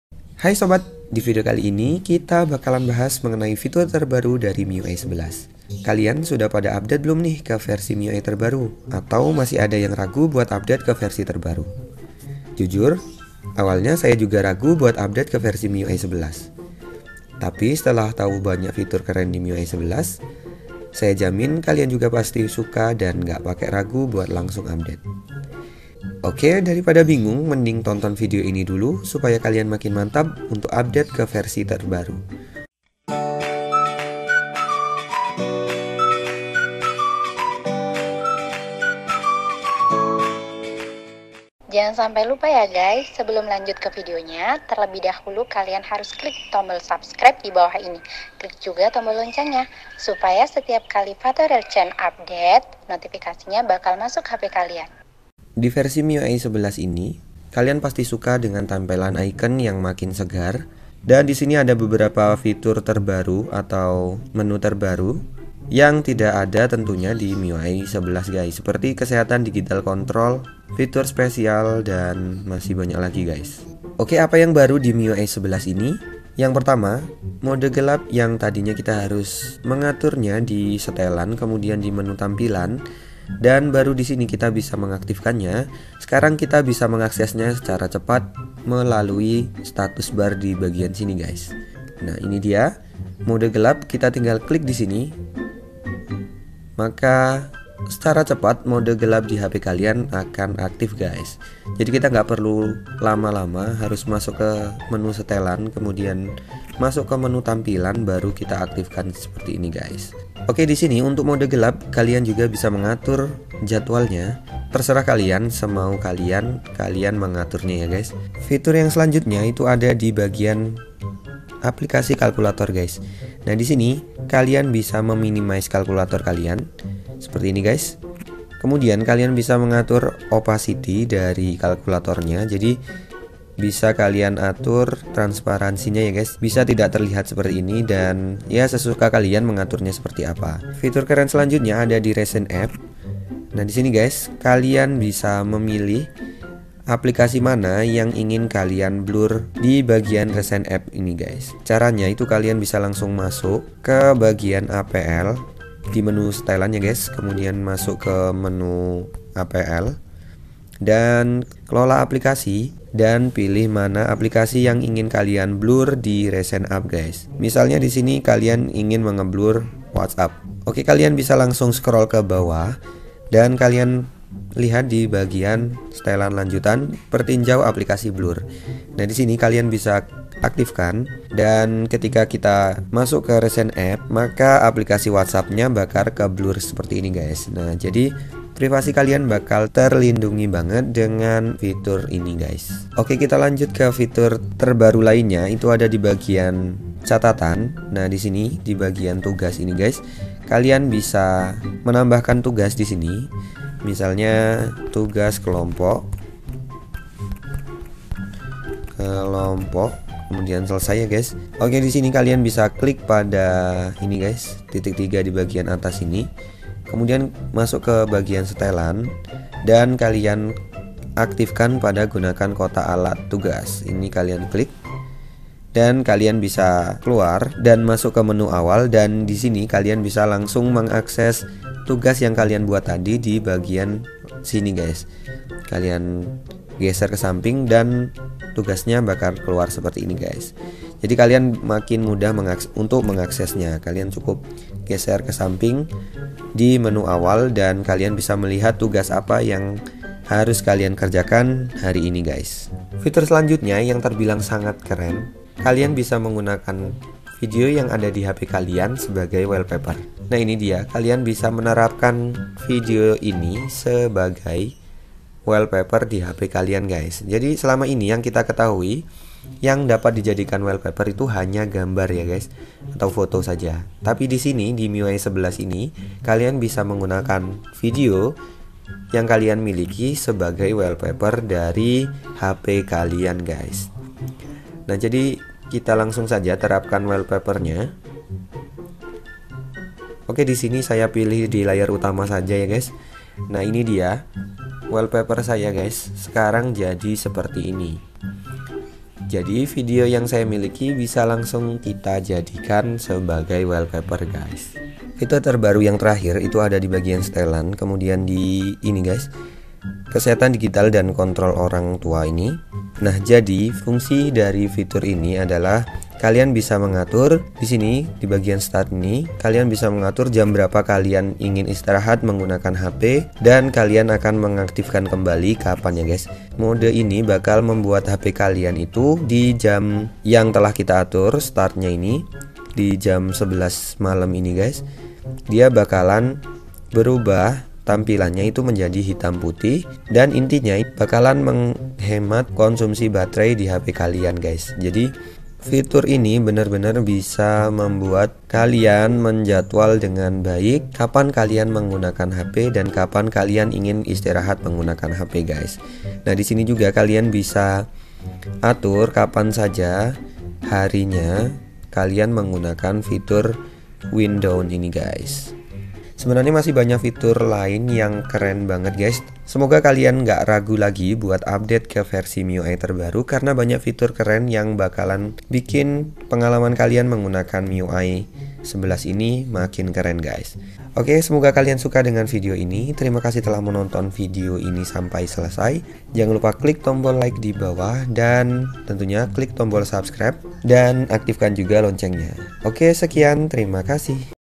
5 FITUR KEREN MIUI 11 INI BAKAL BUAT KAMU NGILERRR🤤
Hi sobat ini adalah 5 Fitur Keren MIUI 11 Yang Wajib Kamu Ketahui ! BURUAN UPDATE VERSI MIUI MU !!!